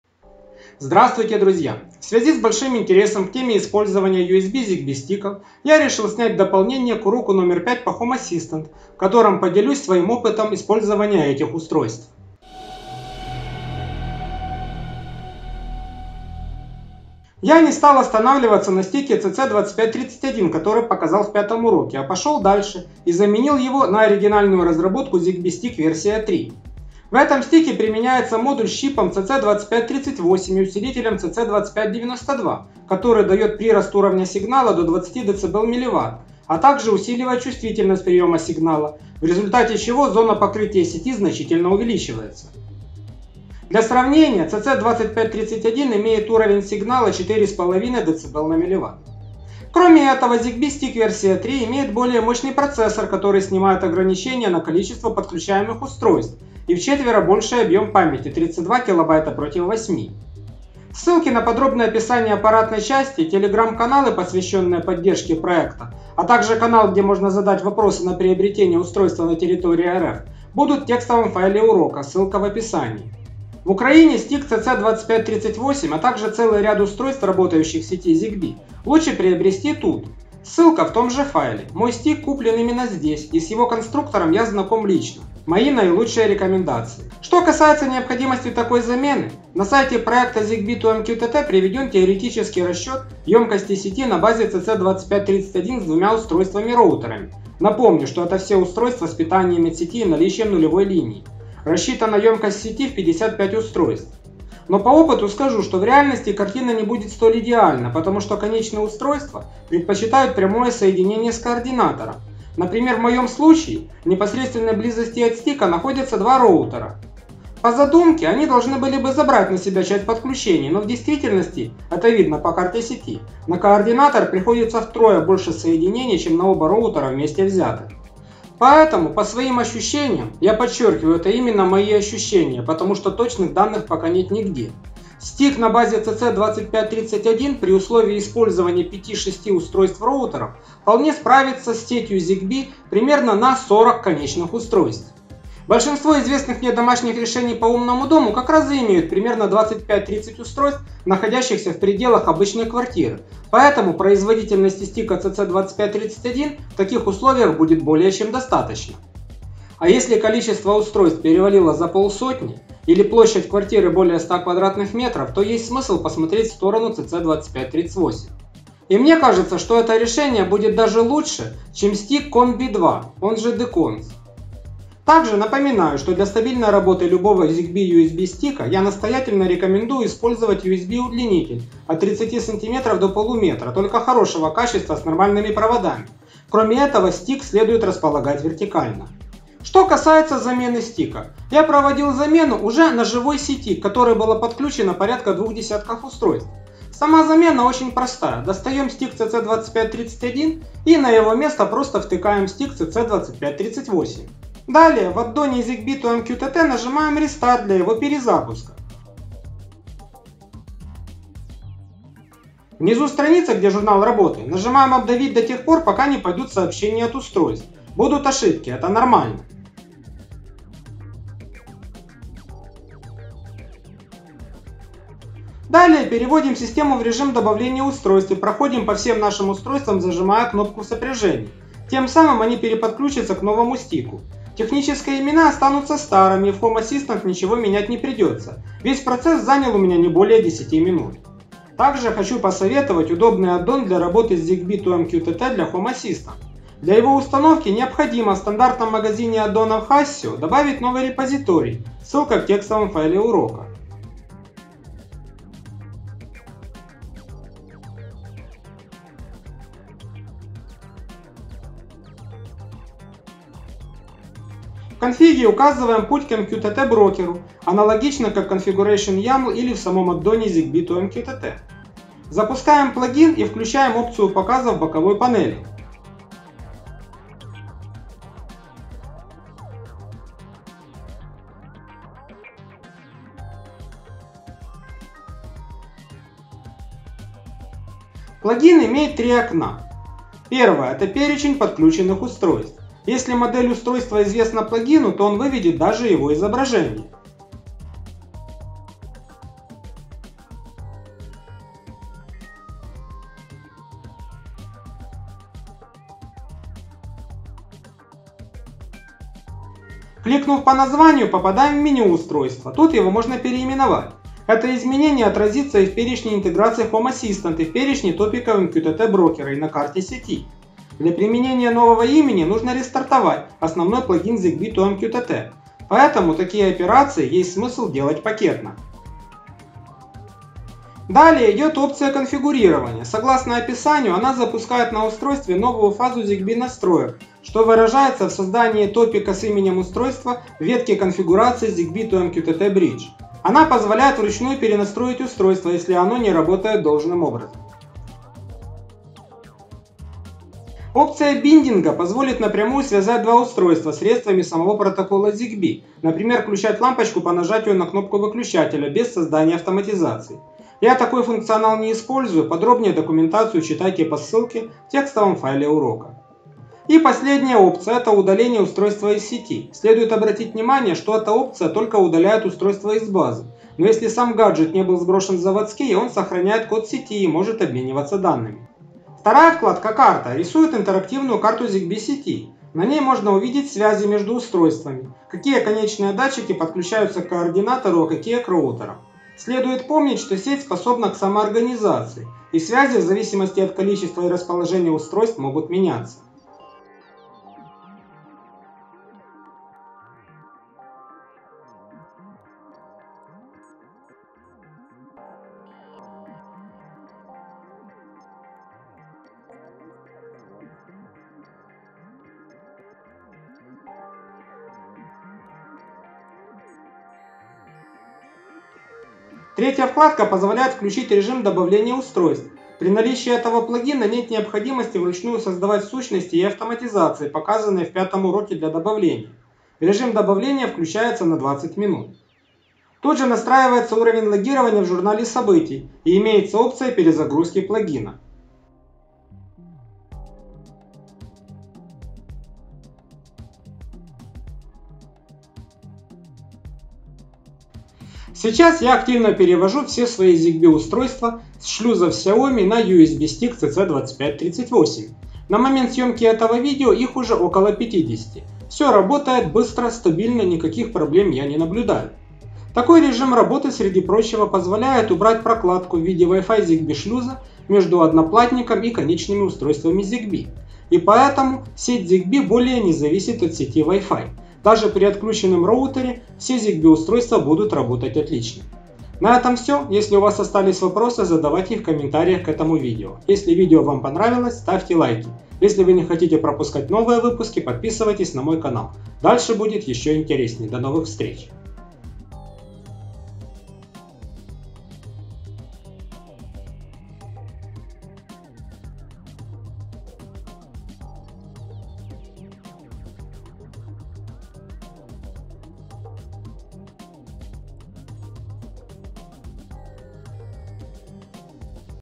Home Assistant. АРХИВ Урок 5.2 USB Zigbee стик CC2538, замена CC2531, аддон Zigbee2MqttAssistant
❗ВИДЕО УСТАРЕЛО ❗

В связи с большим интересом к теме использования USB Zigbee координаторов, я решил снять  дополнение к уроку 5 по Home Assistant, в котором поделюсь своим опытом работы с этими решениями.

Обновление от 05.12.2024
🎞 Обзоры по данной теме: